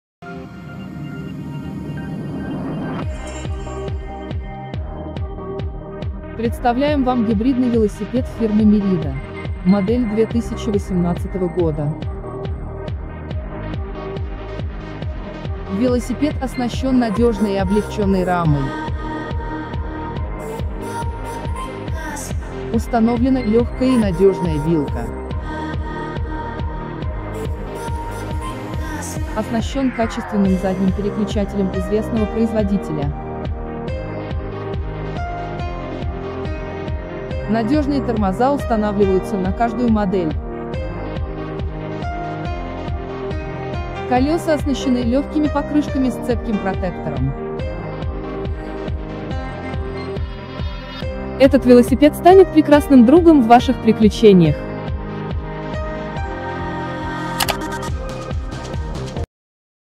Обзор велосипеда MERIDA ESPRESSO 900 2018
Обзор велосипеда MERIDA ESPRESSO 900 2018 модельного ряда MERIDA 2018. Подробнее

Какие особенности данной модели MERIDA ESPRESSO 900, характеристики велосипеда и отзывы о МЕРИДА, кому подойдет и где эксплуатировать этот велосипед, стоит ли купить и какова цена на сайте? Все Ответы смотрите в нашем видео обзоре! Понравилось!?

Основные характеристики велосипеда MERIDA ESPRESSO 900 2018
00:13 Рама: eSPRESSO E8000
00:21 Вилка: Suntour NCX-D-DS-RL, Air fork, 15mm bolt through, CTS, 63mm travel, lockout
00:27 Задний переключатель: Shimano XT M8000 Shadow+
00:35 Тормоза: Shimano XT hydraulic disc brakes with Ice-tech cooling, 180mm rotors
00:42 Покрышки: Continental Ride Tour 42c with reflective strip